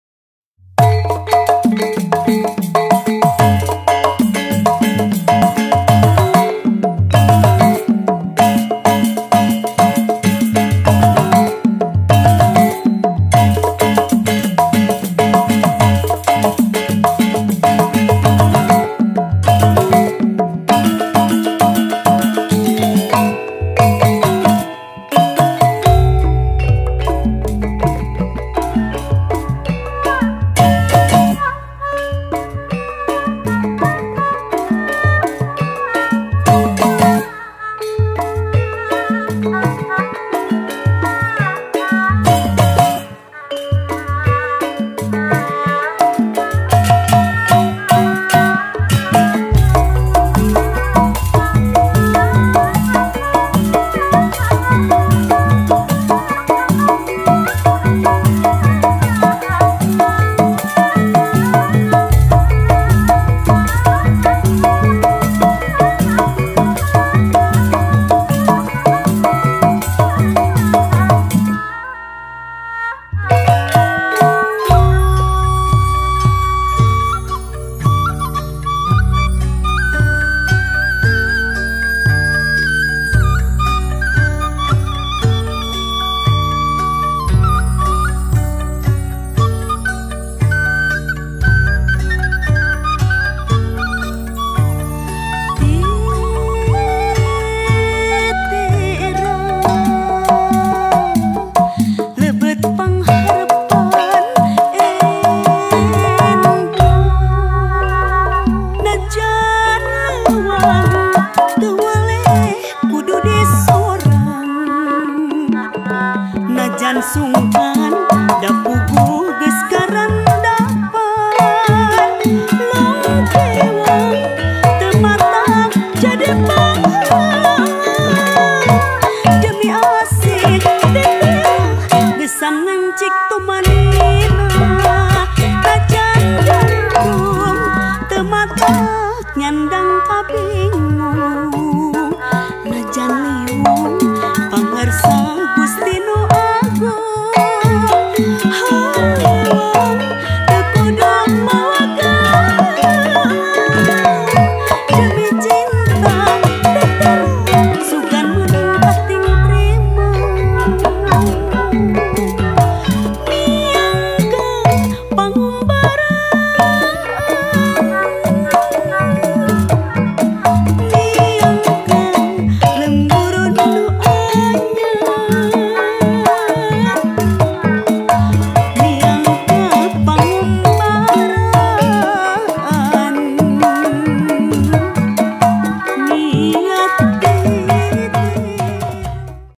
Alfiyanto Wajiwa, Silat WaJiWa
Hap Ta Has Ti,
Wajiwa Junior dalam Pertunjukan  Public Stage Triwulan #10 Rumah Wajiwa